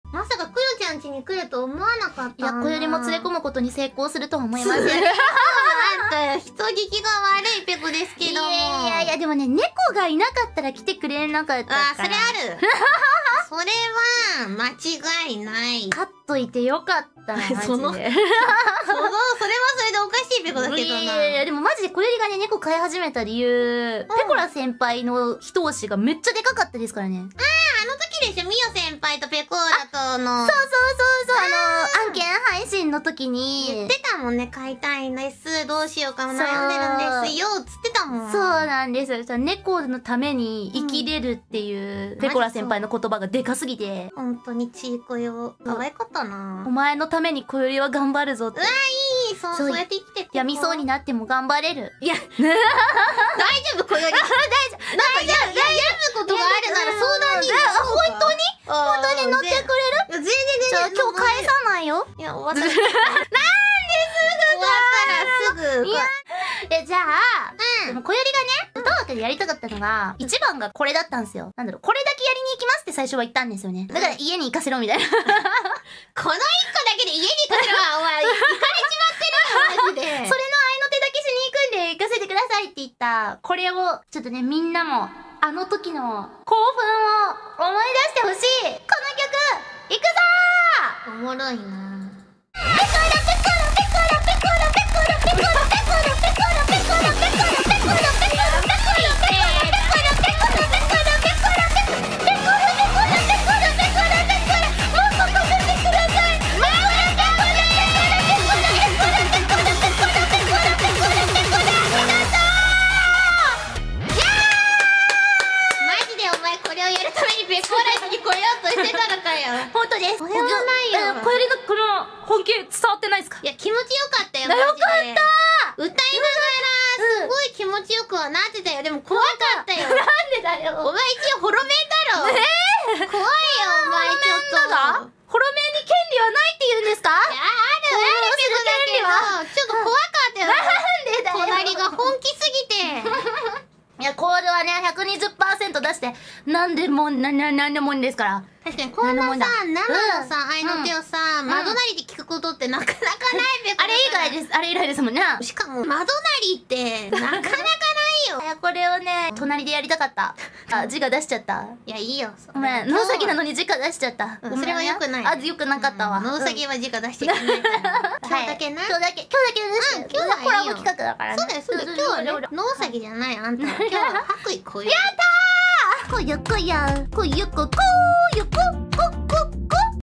【Hololive】Koyori直接在Pekora旁邊熱情打Call 讓兔兔族長有點嚇到【博衣小夜璃/博衣こより/兎田ぺこら/兔田佩可拉】【ホロライブ切り抜き】
元動画
【オフコラボ歌枠】一週間企画5日目！ぺっこよKARAOKEフェス inこよりハウス【ホロライブ/兎田ぺこら/博衣こより」